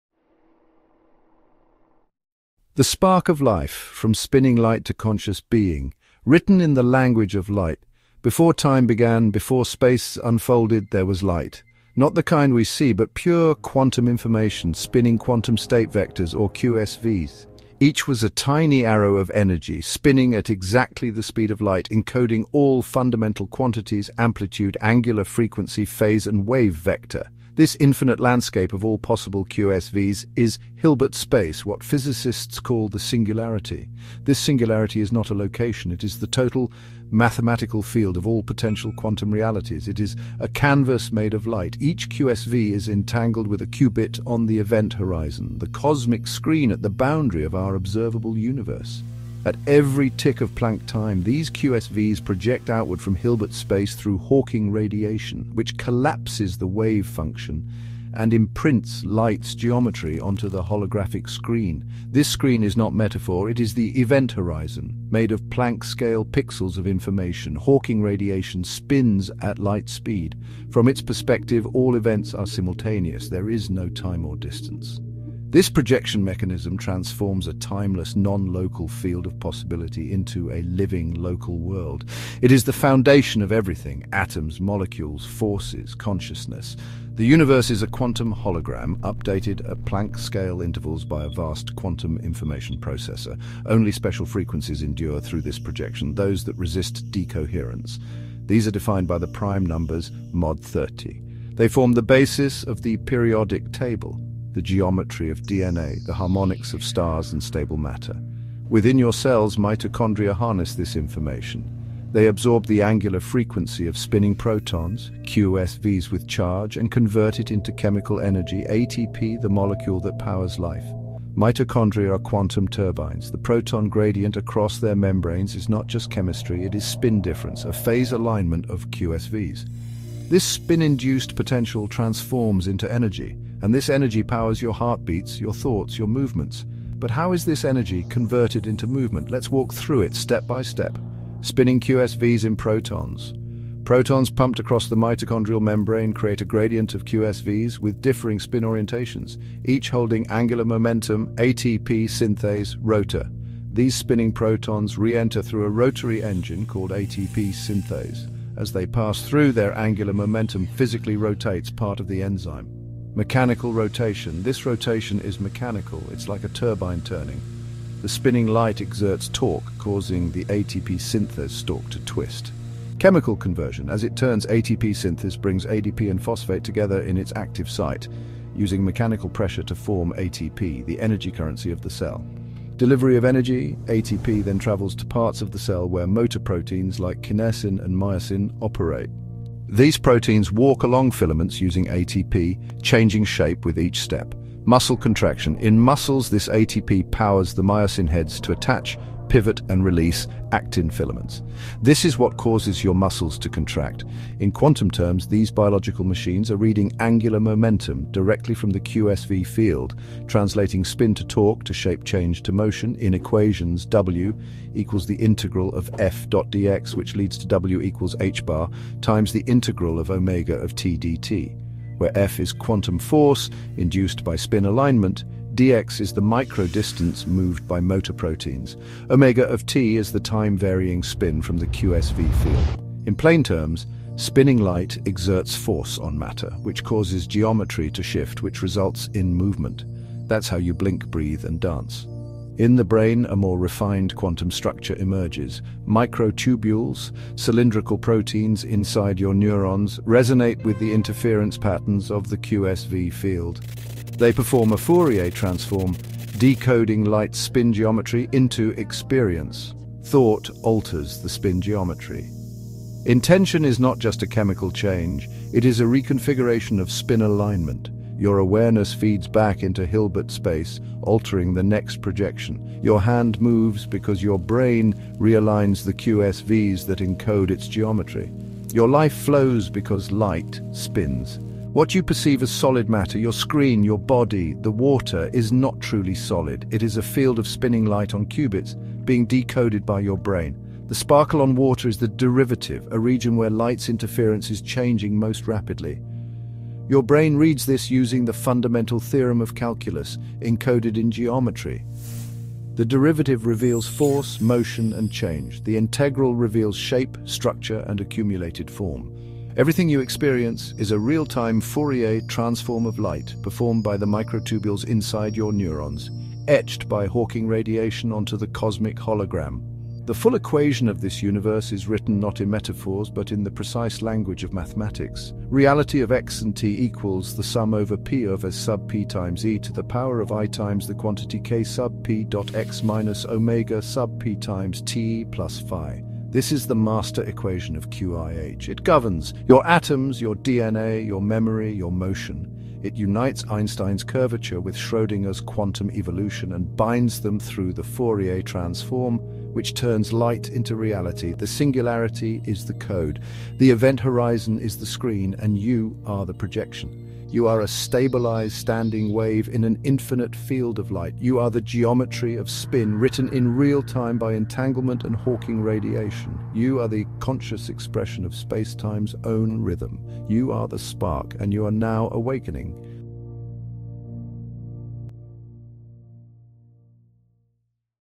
How Light Becomes Life
You are not made of matter.
You are light—spinning at the speed of light.

Each point of your body is a projection of spinning Quantum State Vectors—QSVs—encoded in Hilbert space and etched onto a holographic screen, one Planck moment at a time.

Inside your cells, mitochondria don’t just process food—they decode the angular frequency of light into usable energy:
E=ℏω
E=ℏω

Every heartbeat, every breath, every thought—is light in motion.

Your brain’s microtubules are not just support beams.
They’re quantum processors performing a Fourier transform—translating spinning light into awareness.

The universe is a quantum computer. You are its code.

You are not observing the projection.

You are the projection.

You are the spark.
And you are now awakening.